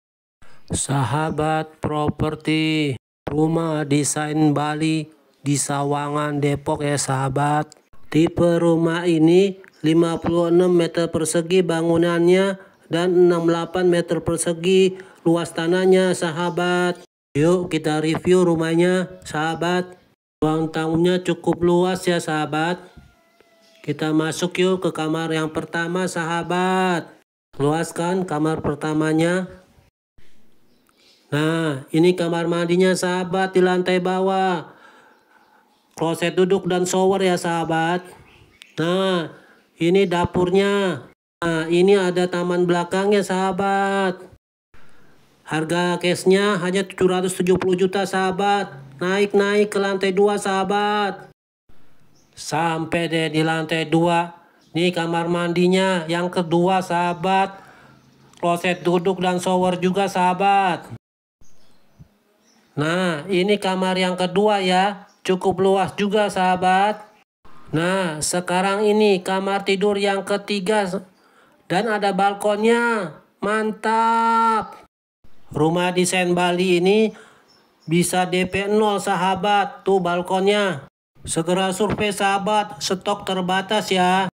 Rumah Dijual Design Bali 2 Lantai Sawangan Depok
Rumah 2 Lantai Dp 0% Akses Terdekat ke Jaksel

Rumah Modern Cluster berlokasi di Cinangka Sawangan dengan akses strategis dekat kemana mana. Dipadu dengan konsep Tropical Eco Living, Minimalis bergaya Bali Modern.

Spesifikasi:
- Luas Bangunan 56m2 / Luas Tanah 68 m2
- Kamar Tidur 3 / Kamar Mandi 2
- Carport, Kitchen, Taman Depan, Guest Room
- Fasum: Mushola, One Gate Sistem

Promo :
DP 0% (CUKUP BF 5JT)
BIAYA AJB, BBN, NOTARIS
BPHTB
BIAYA KPR
BONUS MESIN AIR & TOREN
Bonus Pilihan : AC 1/2 PK/ TV LED

Keunggulan Lokasi:
- The Park Sawangan Depok
- Dekat RSUD Kota Depok
- Dekat Pondok Cabe, Gaplek, Parung, Pamulang
- 8 Menit Terminal Pondok Cabe
- Dekat Jalan Tol Desari Tol Serpong
- Dekat Masjid Kubah Mas
Sertifikat SHM, IMB

Survey Lokasi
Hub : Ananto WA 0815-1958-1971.